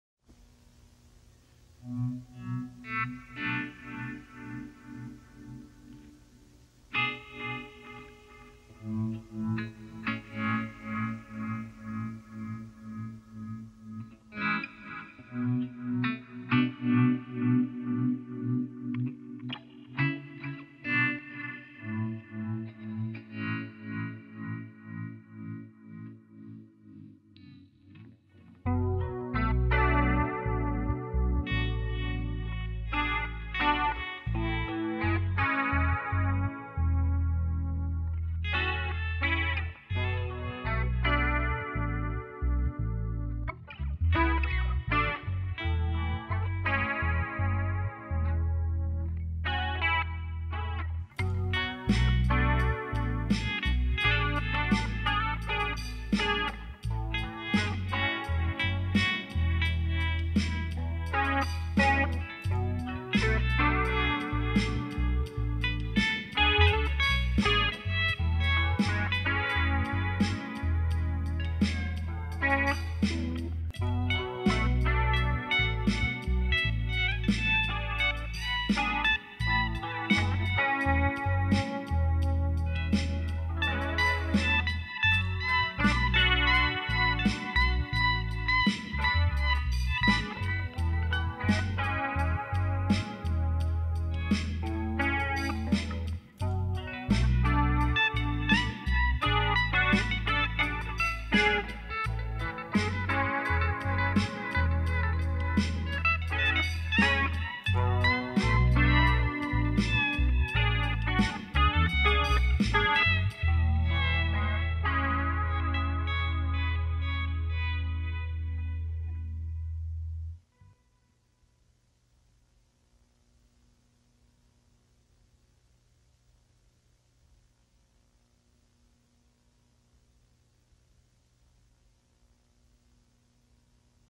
Callejoncito Sounds
Vídeo realizado en la playa de El Callejoncito (Garafia) para el músico Palmero Ramón Gómez Barreto.